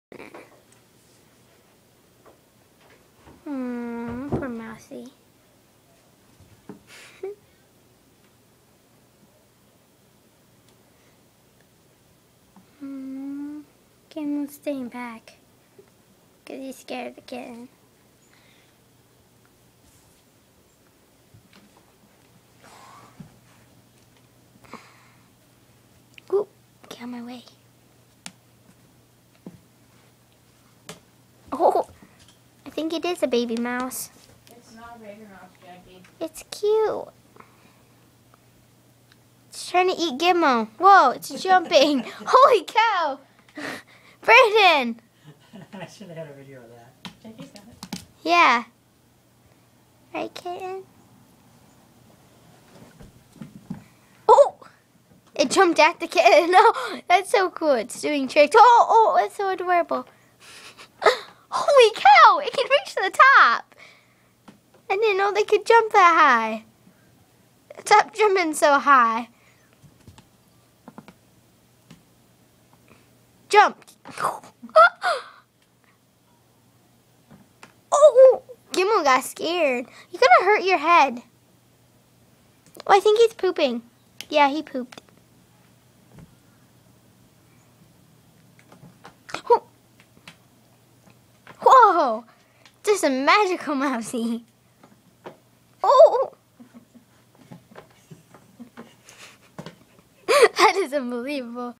High jumping mouse
This mouse gets chased by a cat in a box and jumps really high